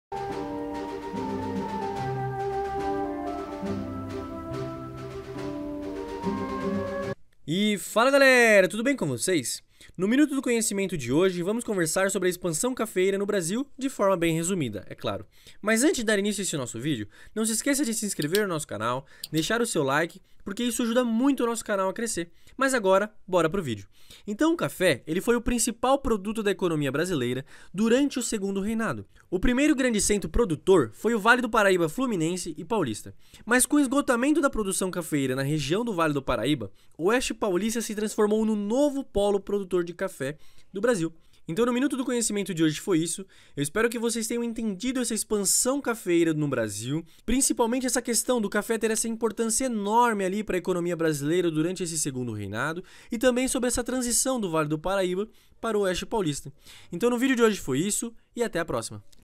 A Expansão Cafeeira No Brasil |Brio Educação
No Minuto de hoje vamos conhecer um pouquinho da expansão cafeeira pelo território brasileiro.
E aí, borá lá? 💡